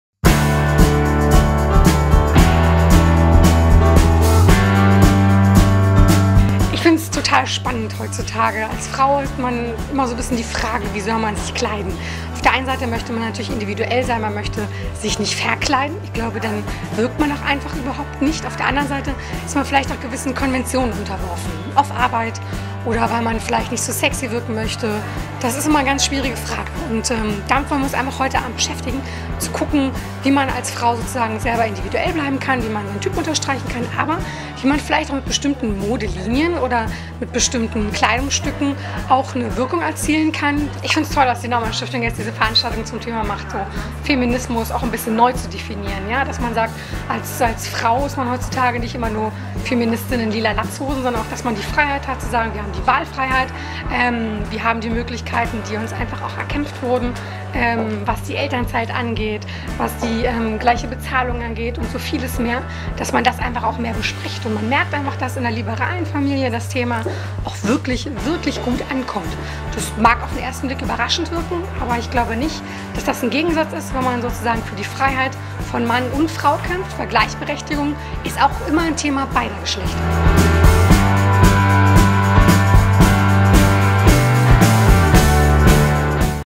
Julia Hesse: Gleichberechtigung ist immer ein Thema beider Geschlechter
Im Rahmen der Veranstaltung "Macht der Mode - Mode der Macht: Power Dressing" am 3. Mai 2016 in Berlin.

Frauenzeitschriften und Online-Ratgeber überschlagen sich mit Tipps, wie man sich als Frau „angemessen“ kleiden sollte: Adrett und schick sollte es sein, aber bitte nicht overdressed und der Rock darf niemals zu kurz oder der Ausschnitt zu tief sein. Stimmt das alles oder es ist einfach egal? Angela Merkel ist das beste Beispiel dafür, dass Macht ganz klar mit Aussehen verbunden ist. Zumindest bei Frauen. Doch wie kleidet man sich, um den eigenen Erfolg zu unterstreichen und gleichzeitig authentisch zu bleiben?

Mit Hilfe einer kompetenten Trainerin sprechen wir darüber, welcher Zusammenhang zwischen Mode und Macht, Wirkung und Ausstrahlung besteht und wie man sich in Politik und Beruf gut kleidet, ohne sich zu verkleiden. Nach einem kurzen Vortrag gehen wir in medias res: Bringen Sie Ihr liebstes Businessoutfit mit und lassen Sie es von unserer Beraterin beurteilen. Oder Sie schauen direkt vor Ort, welches Outfit von den angebotenen für Sie das passende wäre.

Anschließend wollen bei Drinks und Snacks ins Gespräch zum Thema kommen.

19:00 Uhr Grußwort
Der Auftritt als Frau in der Politik
Dr. Maren Jasper-Winter, Freie Demokraten Berlin

19:15 Uhr Impulsvortrag
Macht der Mode – Mode der Macht
Katrin Sachs, Trainerin

19:45 Uhr
Power Dressing: Sie sind gefragt: Suchen Sie Outfits oder ziehen Sie das mitgebrachte Outfit an!

20:15 Uhr
Beratung und praktische Tipps zu den Outfits von der Expertin und den anderen Teilnehmerinnen

21:15 Uhr
Get-Together mit Drinks und Snacks